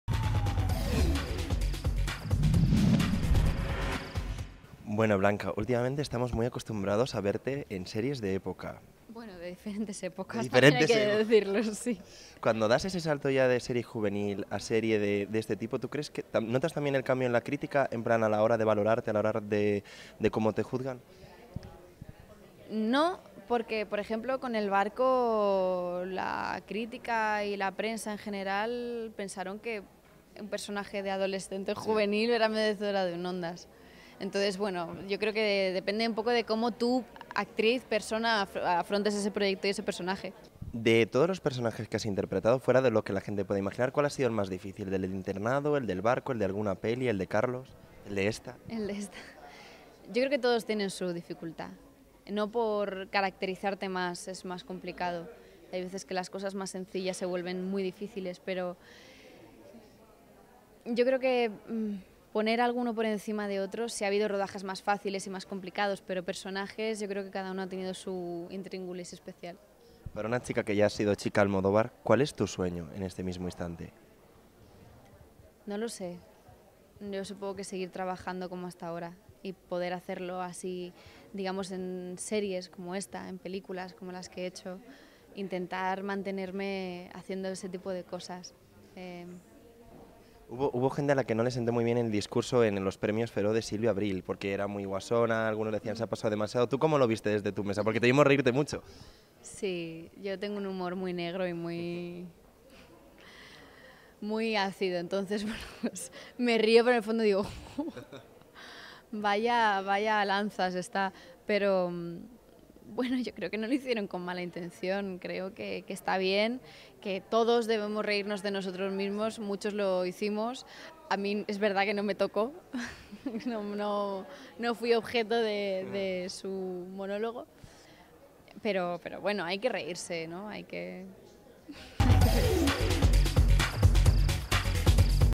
¿Siente Blanca Suárez que la valoran más ahora que cuando hacía series juveniles?
Blanca Suárez dice lo que piensa del monólogo de Silvia Abril en los Premios Feroz.